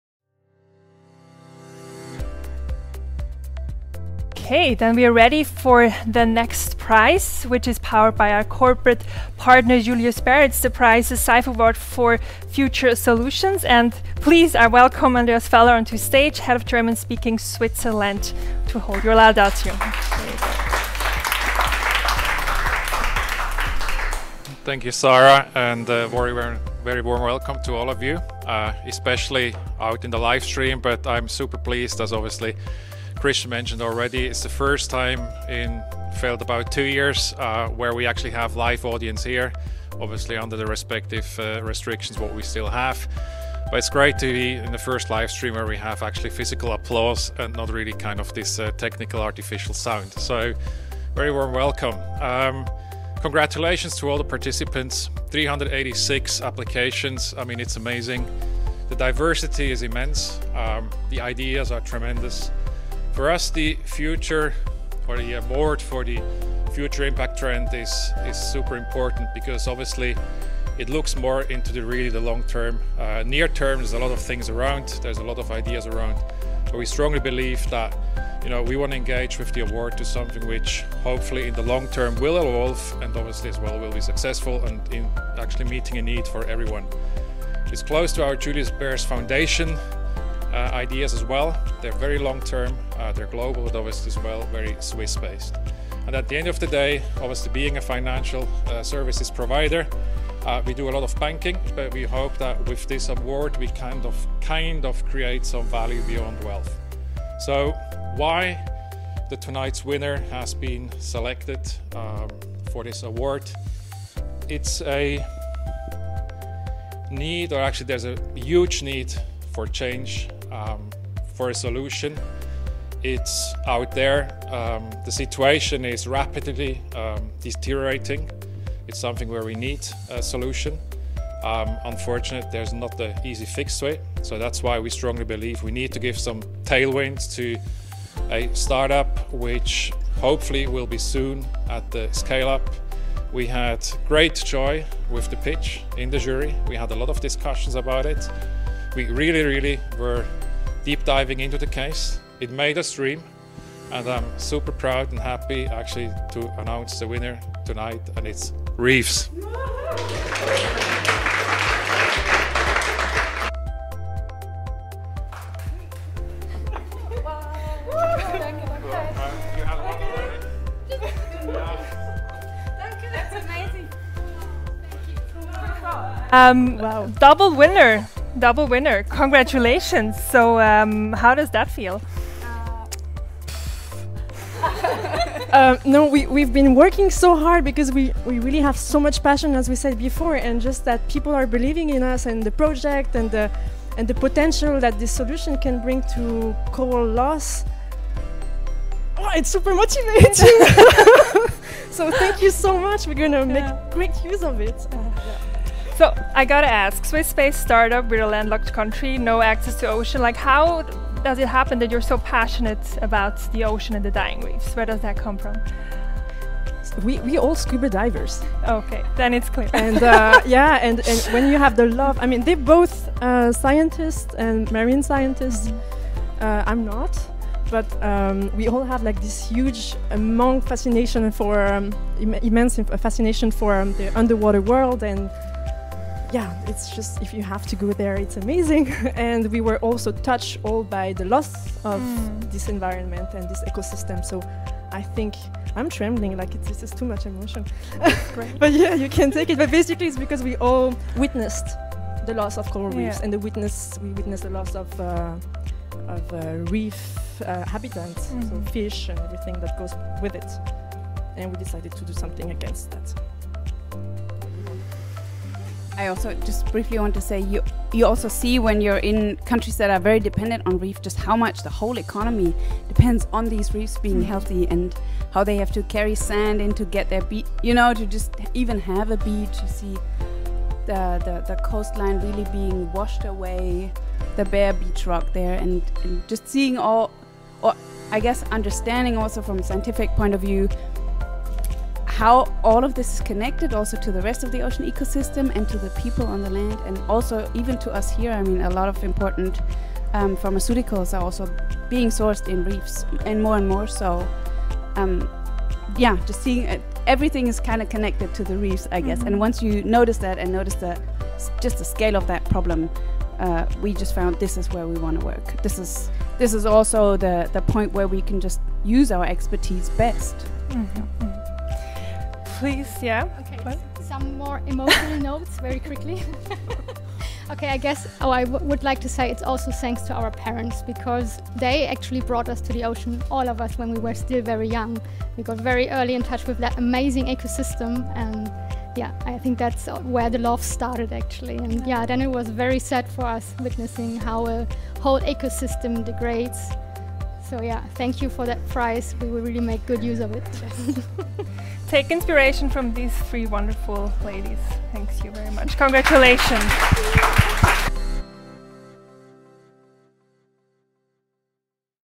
Julius Bär Award for Future Impact Trend
Winner: rrreefs
Speaker: Sarah Linder, Managing Partner SEIF / Andreas Feller, Head German-Speaking Switzerland, Julius Bär / Dr. Ulrike Pfreundt, Marie Griesmar, Hanna Kuhfuss, co-founders rrreefs